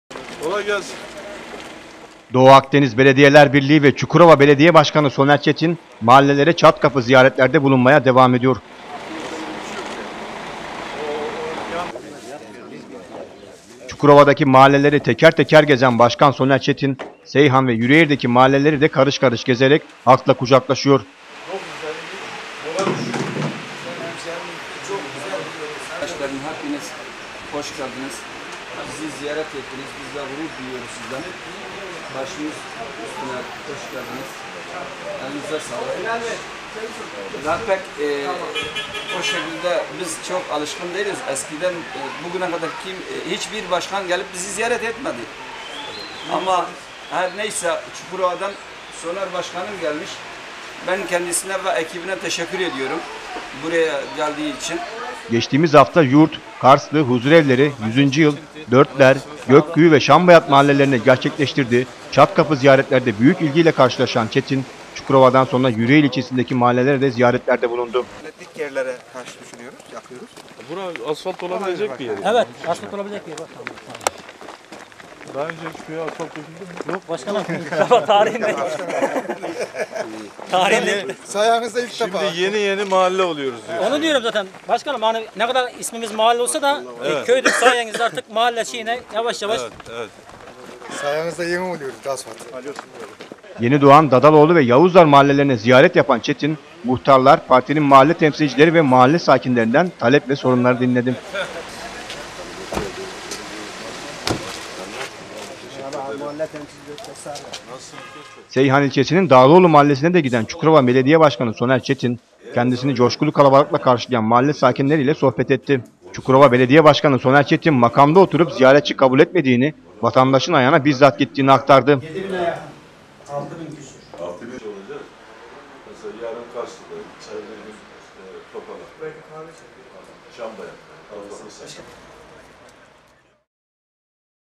SONER ÇETİN HALKLA BULUŞTU
Çukurova Belediye Başkanı Soner Çetin halkla buluştu.